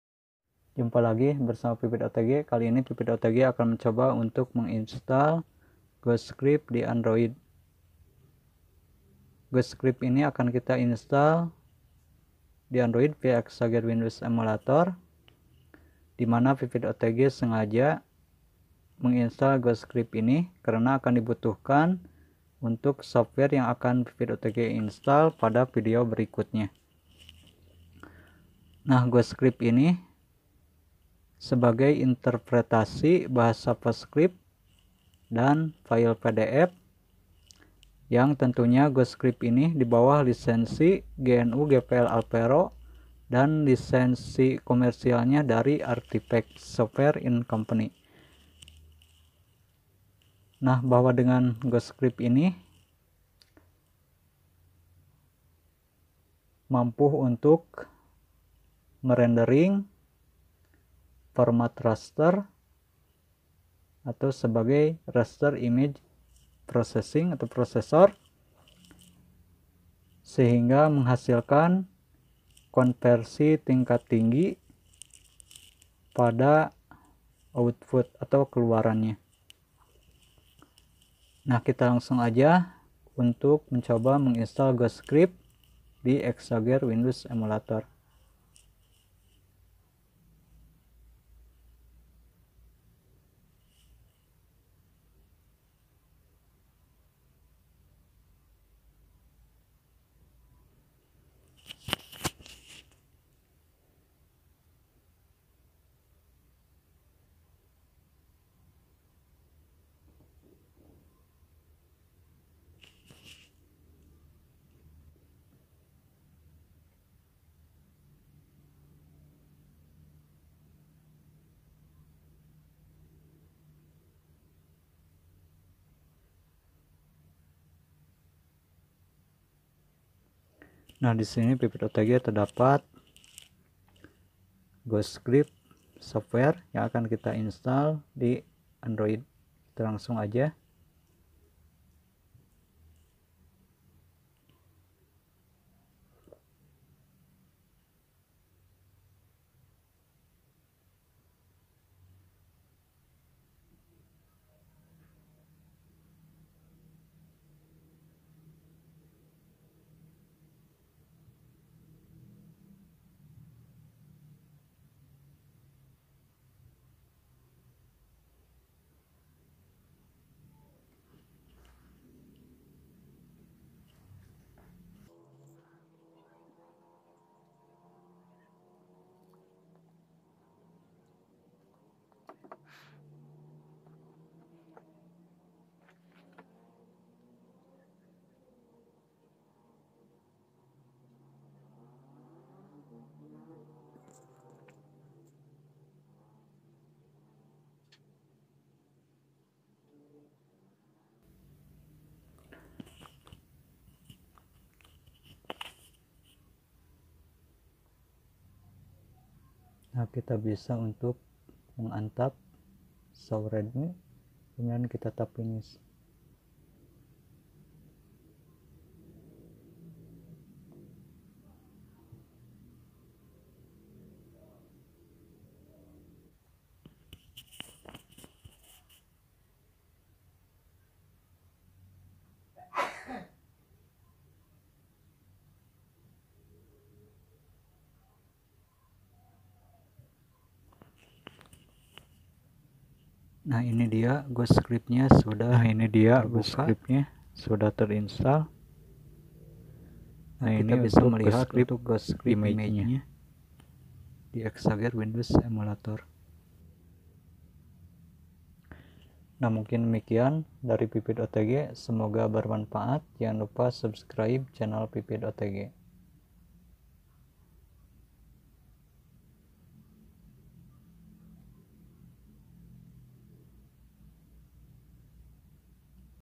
Instal GhostScript Di Exagear Windows Emulator Android
Ghost Script sebagai interpreterer PostScript dan juga file pdf.

Ghos script software ini mampu menghasilkan output vektor berkualitas tinggi dan bagus sebagai RIP (Raster Image Processing).